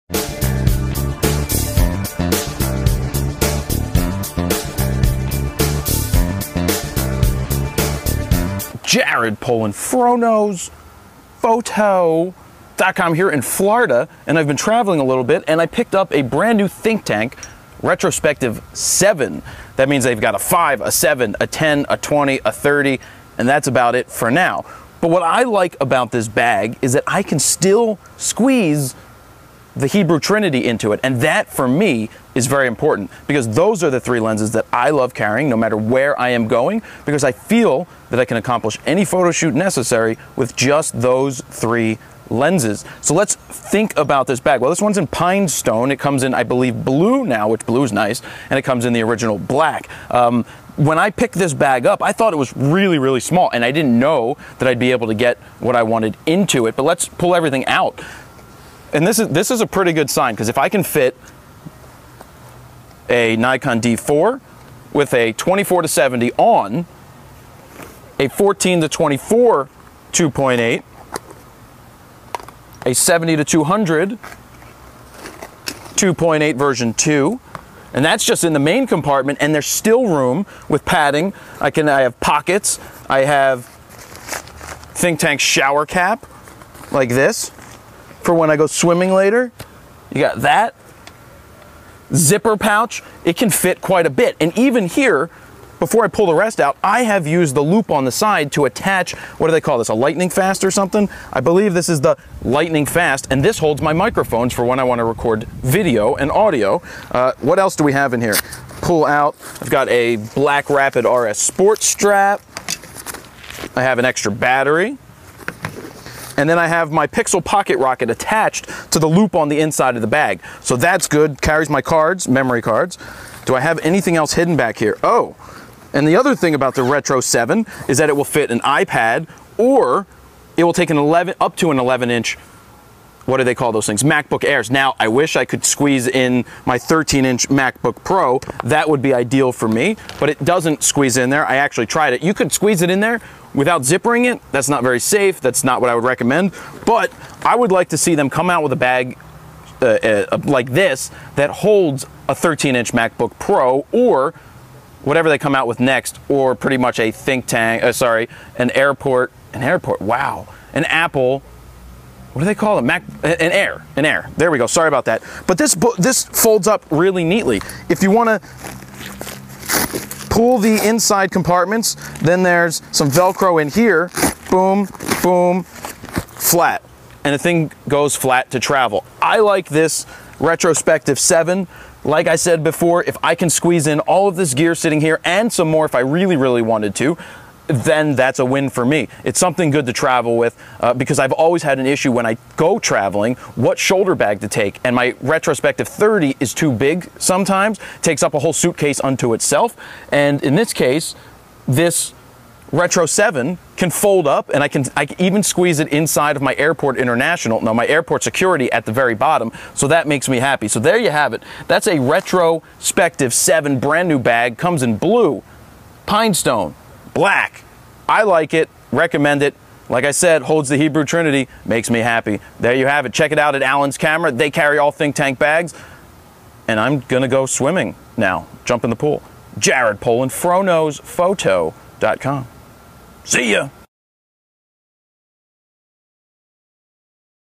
ThinkTank Photo Retrospective 7 Bag Review
Recently ThinkTank Photo released the Retrospective 7 shoulder bag.  I currently use the Retrospective 30 as my main go to shoulder bag on location photo shoots.  What I personally look for in a bag is can it fit the hebrew trinity and is it built well.

First things first, this bag is built extremely well like all other ThinkTank Bags.  The bags they make are made like no others that I have tried in the past, they are built by photographers for photographers.

Does the bag fit the hebrew trinity?  The answer to that is yes it does.  It fits my Nikon D4, Nikon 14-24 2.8, Nikon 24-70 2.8 and Nikon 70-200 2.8 VRII.  Not only does it fit all of that gear but there is plenty of space for extra batteries, accessories and even and iPAD or Mac Book Air 11 inch.  I wish I could squeeze in my 13 in Mac Book Pro but it just would not fit.

I was very surprised that such a small bag could fit so much and still break down to be so small.  When I am traveling I like to travel light and still have a shoulder bag for photo shoots.  You can not replace a shoulder bag when on a shoot, you can access all of your lenses quick opposed to back packs.  What I like about this bag is I can empty it, flatten it and place it in the bottom of my ThinkTank Airport Security.

All and all this is a great option for a light yet sturdy bag for every day use.  If you are looking for a bag to store one body from a pro body to a basic DSLR, a few lenses and accessories and an iPad this bag could be for you.  Check out AllensCamera.com for all of your ThinkTank Photo Bag needs.